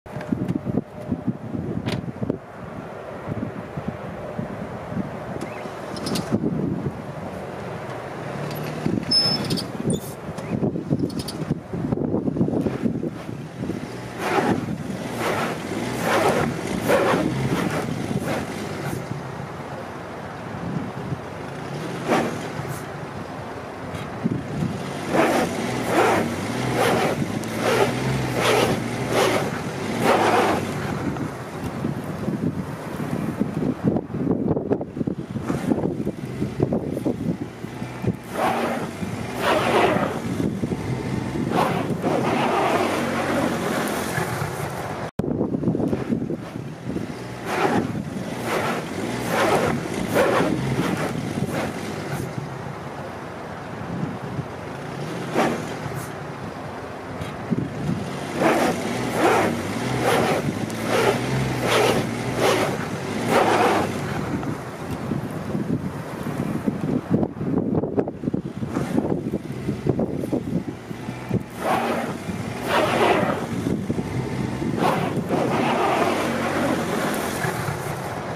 Hot summer BMW spinning rear tires on a gravel hill
I spin my rear tires on a gravel climb way

patinaj çekmek

ca patine